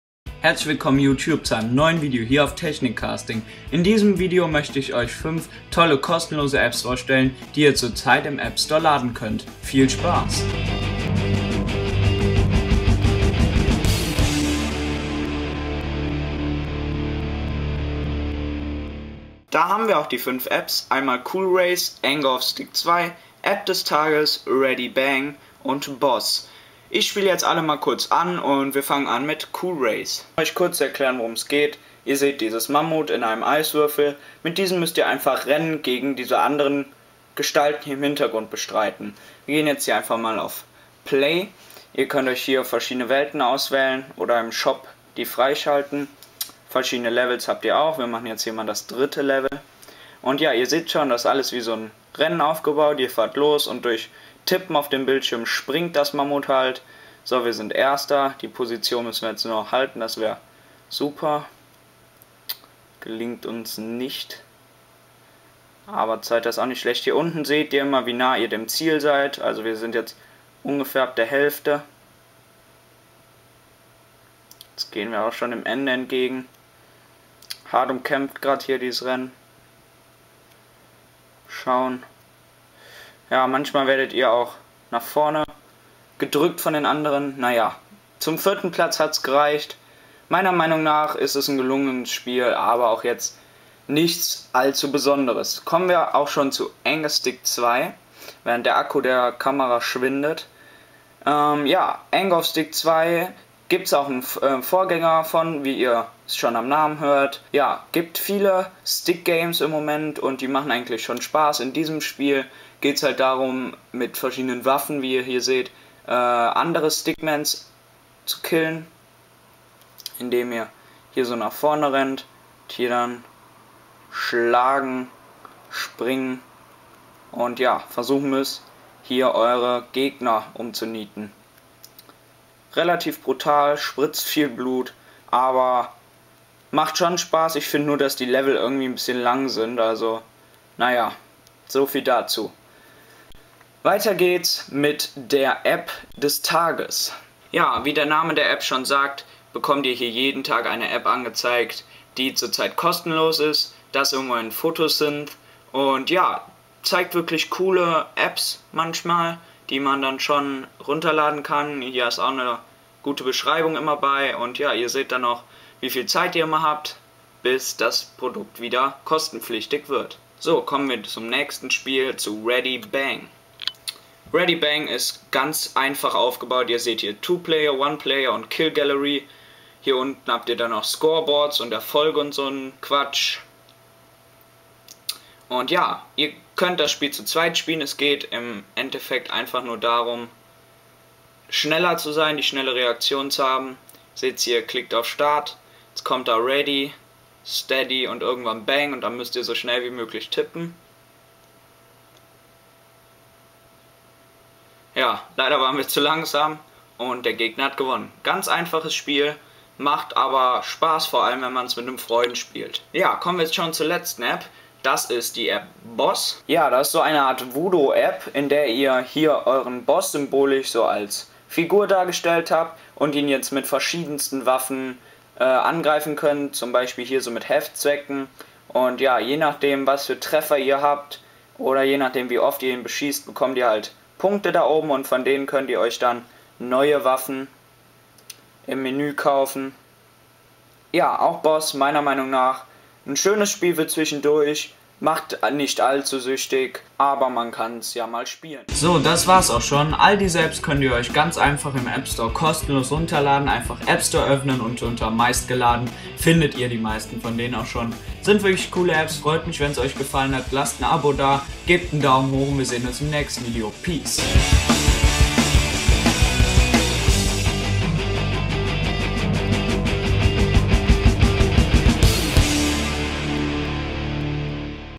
5 Top Apps Kostenlos I iPhone I Deutsch
Hey YouTube,
in diesem Video stelle ich euch 5 Apps vor die ihr zur Zeit kostenlos im AppStore laden könnt. Viel Spaß